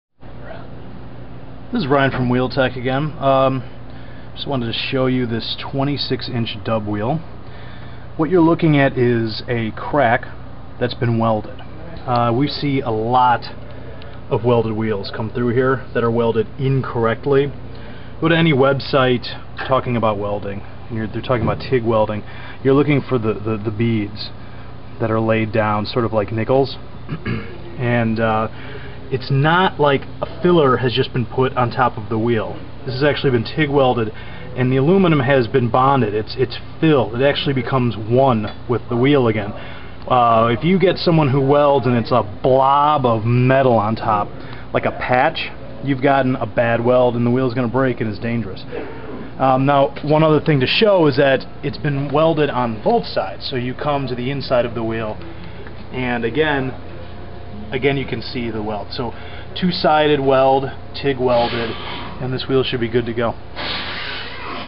Dub Alloy Wheel with Weld
In this video a Wheel-Tech Technician describes how a weld is used to repair a long crack on 1 26" Dub Wheel. He also describes why this particular weld is better than other welds that looks like a patch or "band-aid"